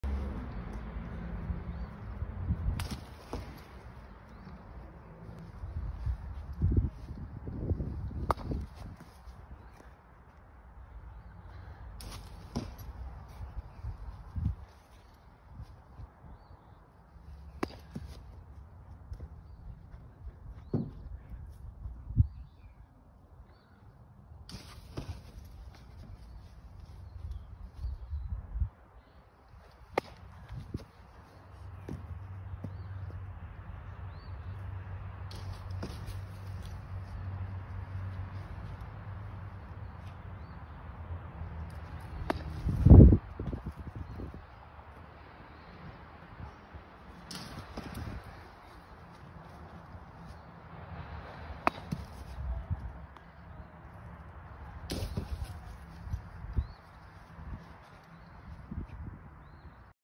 Dawgs Lucas Capozzoli - Fielding
Dirt Dawgs Baseball 10u Purple Lucas Capozzoli working on his fielding.